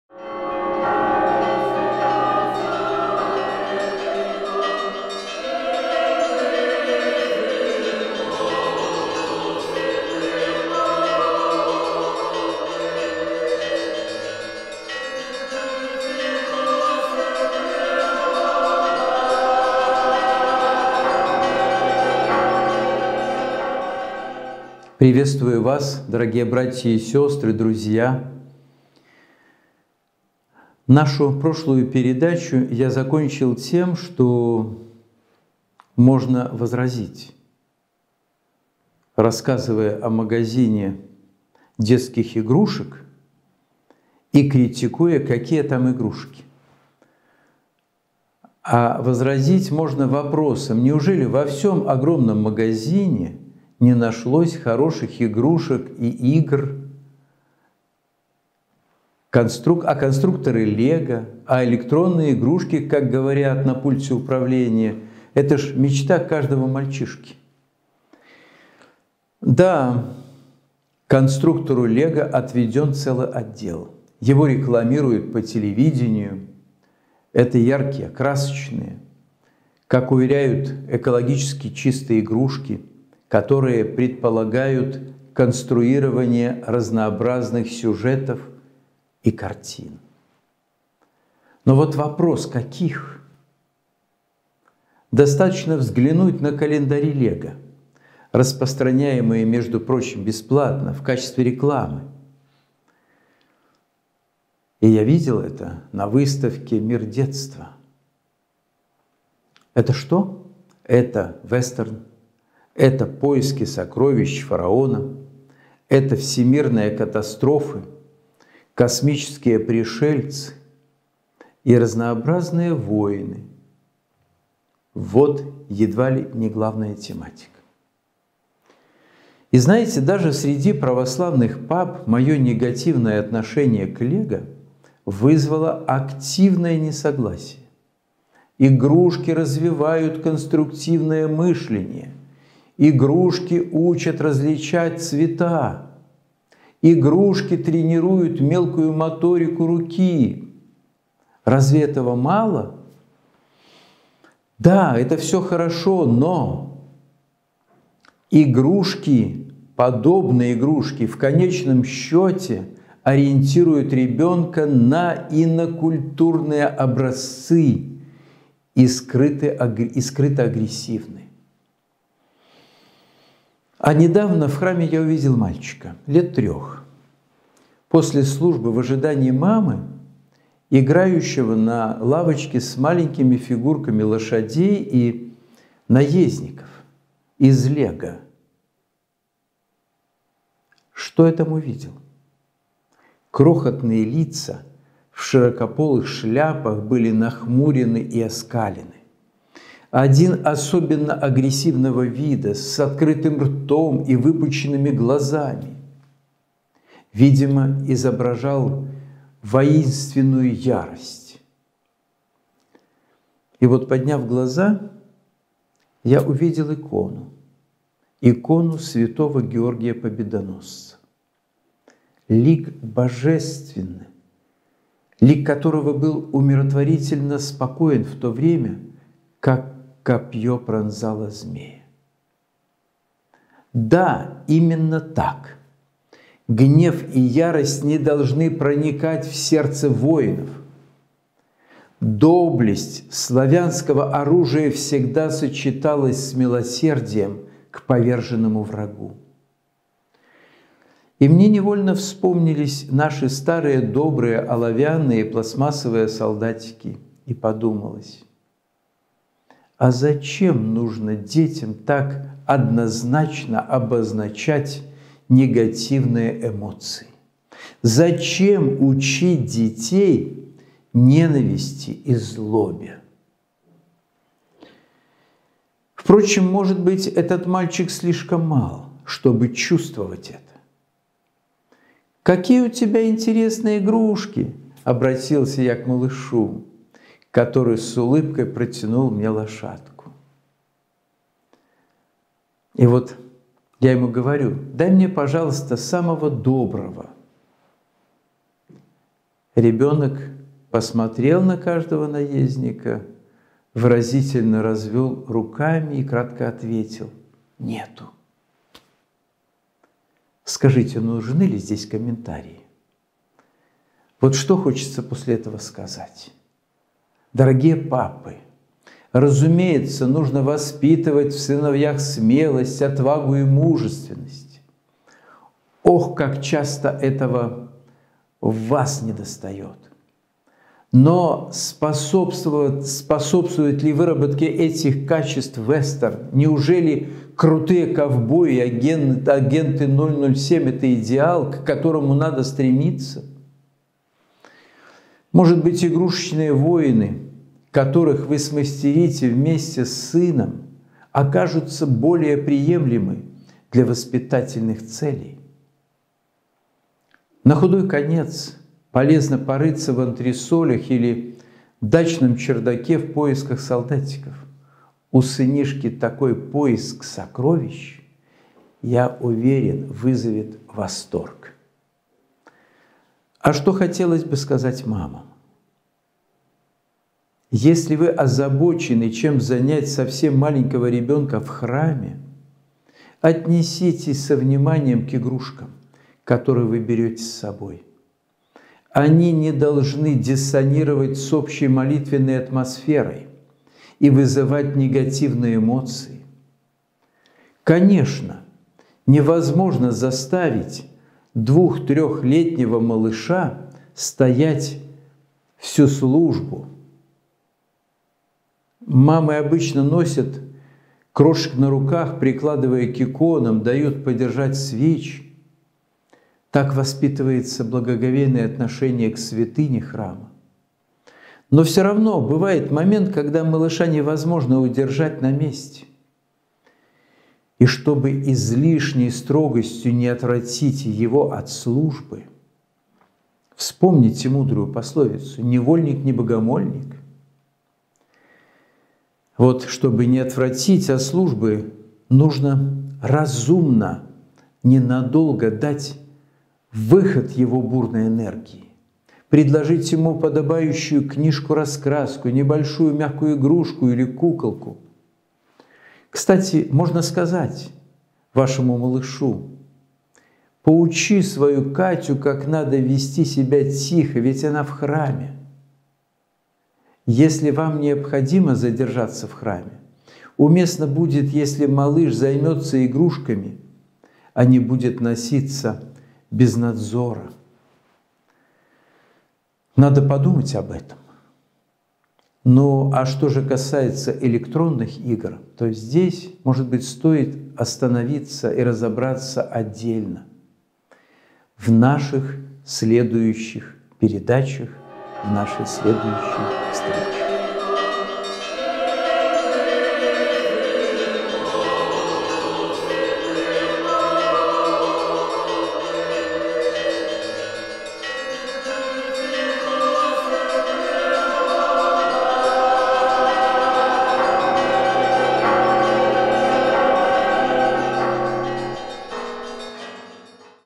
Воскресная проповедь. Так ли Lego безобиден для ребёнка и к какому идеалу надо стремиться?
Поговорим об играх и игрушках, которые окружают детей и влияют на них серьезным образом.

Автор и ведущий: протоиерей Фёдор Повный.